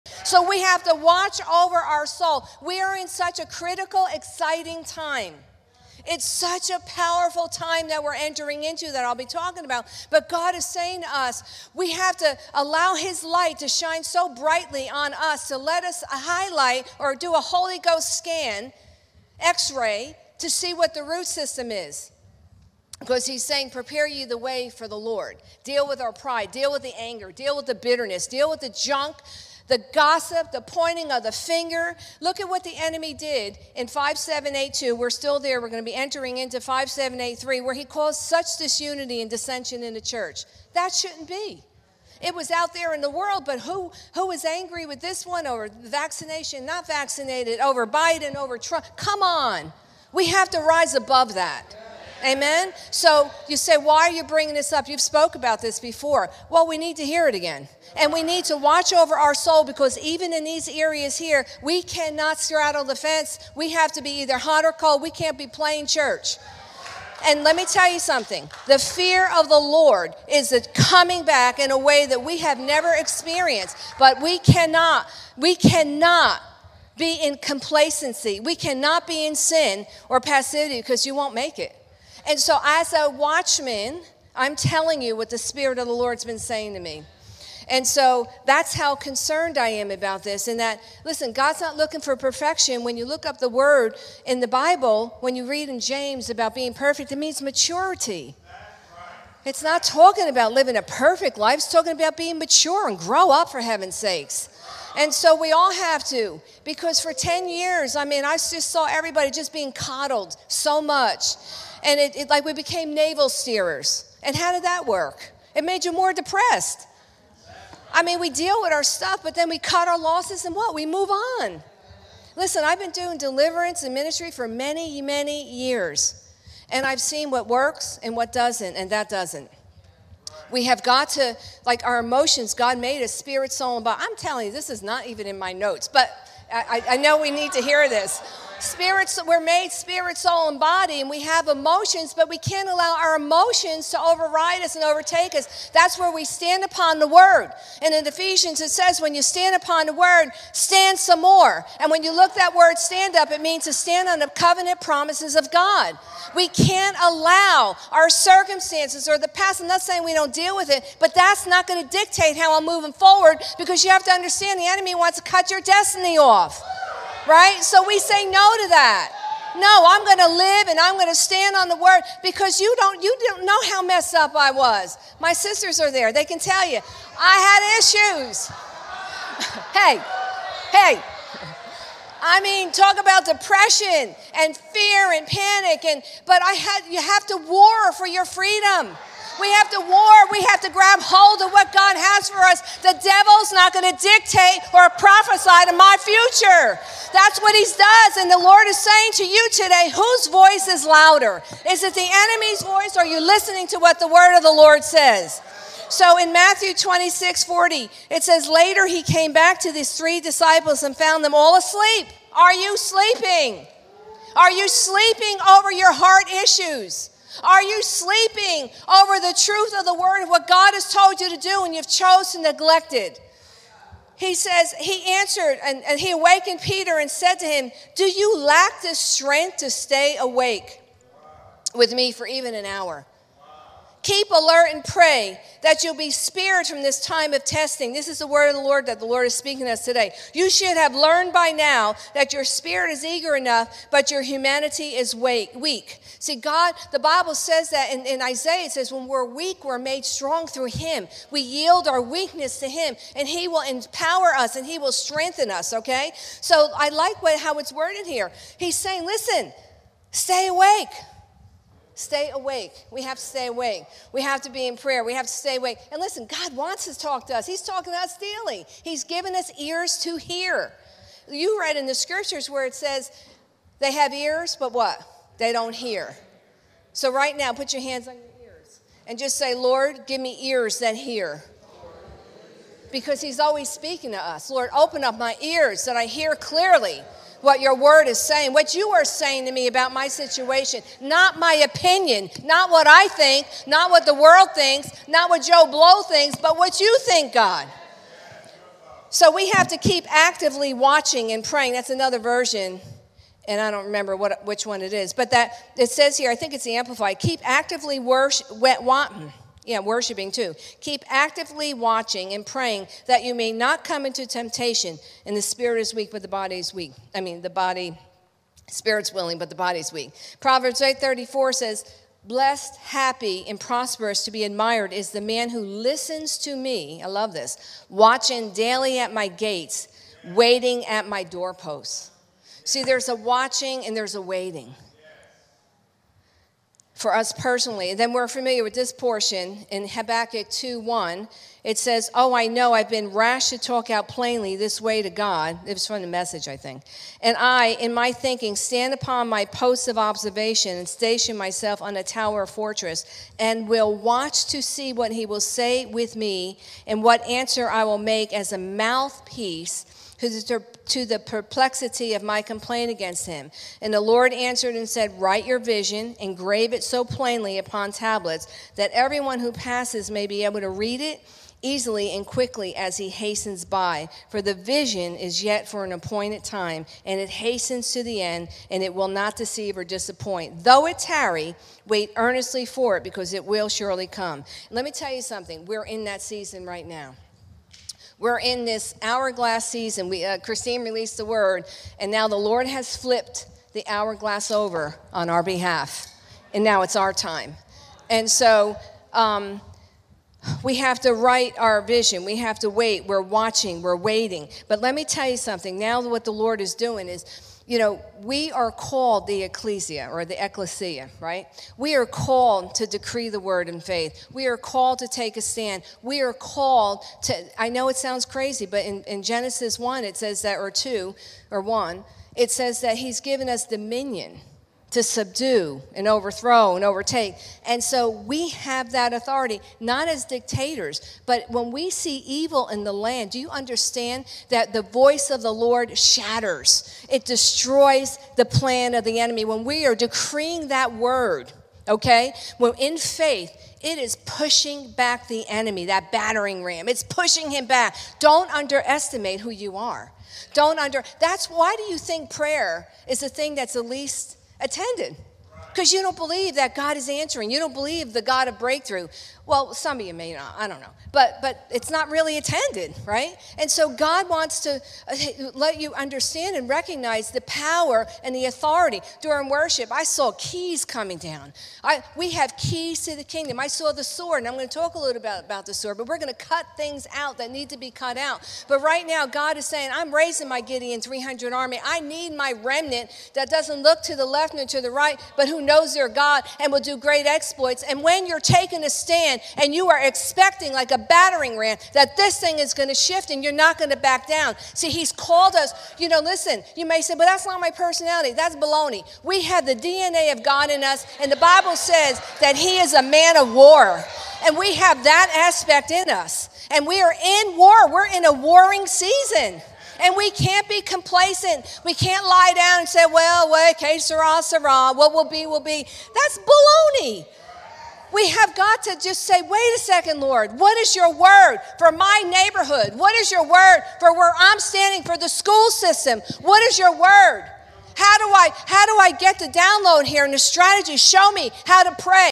Trisha Roselle: Forbid the Enemy to Prophesy Lies Into Your Future (John 8:44)
Trisha speaks at the King of Kings Watchman Conference on 9/9/2022.
Quotes: “You have to war for your freedom. We have to grab hold of what God has for us. The devil's not going to dictate or prophesy into my future. Whose voice is louder? Is it the enemy's voice or are you listening to what the word of the Lord says?”

We're in such a critical and exciting time, so we have to watch over our soul.  We have to allow His light to highlight what our root systems are. He's saying prepare the way for the Lord. It’s time to deal with our pride, the anger, the bitterness, the junk, the gossip, the pointing of the finger.

Look at what the enemy did in 5782 where there was such disunity and dissension in the church. That shouldn't be. We have to rise above that. We need to watch over our soul, not straddle the fence. We have to be either hot or cold. We can't be playing church. The fear of the Lord is it coming back in a way that we have never experienced, but we cannot be in sin or passivity, cause you won't make it. God's not looking for perfection. When you read in James about being perfect it means maturity. It's not talking about living a perfect life, it's talking about being mature and growing up.

I've been doing deliverance ministry for many years. I've seen what works and what doesn't and that doesn't. God gave us emotions, but we can't allow our emotions to override His Word. Ephesians says when you stand upon the Word, stand some more. Stand means to stand on the covenant promises of God.

You didn't know how messed up I was, talk about depression and fear and panic! But you have to war for your freedom. We have to grab hold of what God has for us. The devil's not going to dictate or prophesy to my future. Whose voice is louder? Is it the enemy's voice or you're listening to what the word of the Lord says?

 In Matthew 26:40 it says, "Later he came back to these three disciples and found them all asleep." Are you sleeping, over your heart issues, over the truth of the Word of what God? “He awakened Peter and said to him, do you lack the strength to stay awake with me for even an hour? Keep alert and pray that you'll be spared from this time of testing.” We have to stay awake. We have to be in prayer, we have to stay awake and listen, God wants to talk to us daily.

The Lord is speaking to us today. You should have learned by now that your spirit is eager, but your humanity is weak. The Bible says that in Isaiah it says when we're weak, we're made strong through him. You read in the scriptures where it says they have ears but what? they don't hear. So right now, put your hands on your ears and just say, Lord, give me ears that hear; because he's always speaking to us. Lord, open up my ears so I hear clearly what your word is saying, what you are saying to me about my situation.  Not my opinion, not what the world thinks, what You think. We have to keep actively watching and praying, there’s another version that it says keep actively watching and praying that you...”

Proverbs 8:34 says, 'Blessed, happy and prosperous to be admired is the man who listens to me, I love this watching daily at my gates, waiting at my door post."  See there's a watching and there's a waiting for us personally. In Habakkuk 2:1 it says, “…station myself on a tower of fortress and will watch to see what He will say with me. The Lord said write your vision and engrave it so plainly upon tablets that everyone who passes may be able to read it easily and quickly as he hastens by, for the vision is yet for an appointed time and it hastens to the end, and it will not deceive or disappoint. Though it tarry, wait earnestly for it because it will surely come."

We’re in that season right now.  The Lord has flipped the hourglass over on our behalf, and now it's our time. We have to write our vision. Genesis 1:26 says, He's given us dominion to subdue and overthrow and overtake. We have that authority. We have keys to the kingdom, but we're going to cut things out that need to be cut out.

God is saying, I'm raising my Gideon 300 army.  I need my remnant that doesn't look to the left nor to the right but who knows their God and will do great exploits. We have the DNA of God in us, and the Bible says that He is a man of war. We're in a warring season, and we can't be complacent. We have got to just say, Lord what is Your word for my neighborhood, for the school system? Show me how to pray.